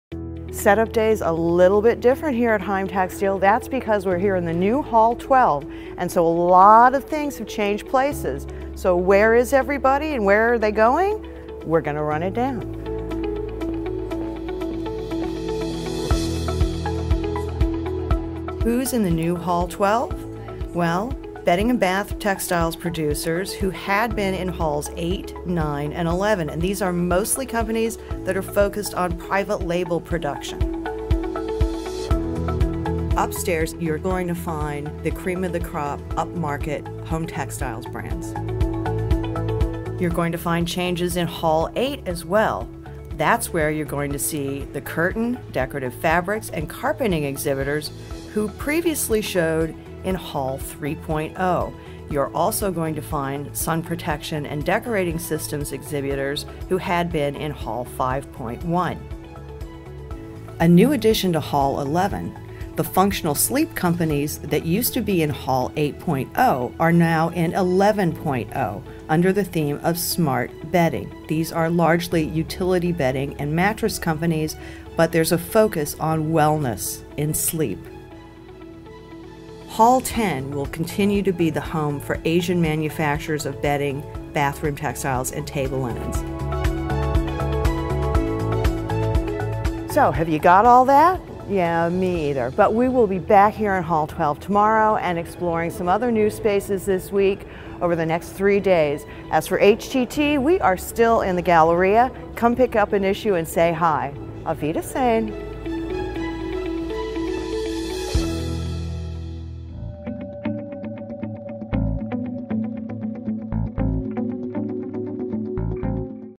Heimtextil Opening Day
With the opening of Hall 12, Heimtextil has reworked the exhibitor layout. HTT editor-in-chief Jennifer Marks highlights the key changes.